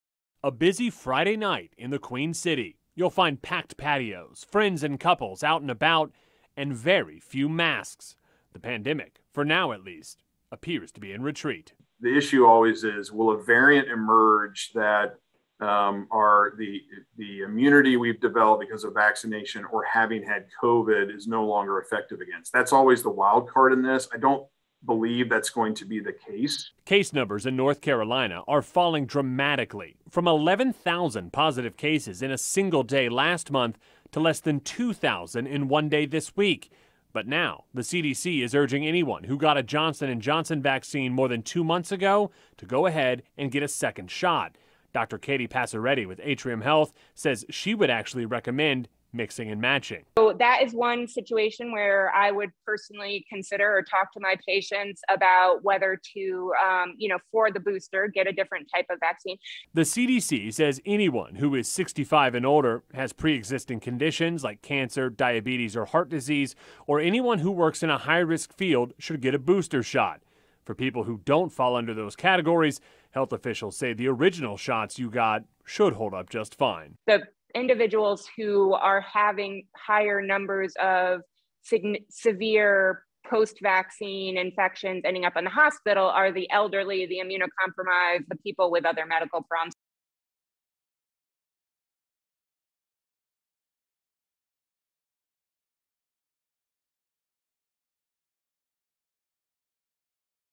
Johnson & Johnson COVID-19 vaccine recipients urged to get a second dose
The nearly 600,000 thousand people in the Carolinas who got a Johnson & Johnson COVID-19 vaccine shot are being urged to get a second dose.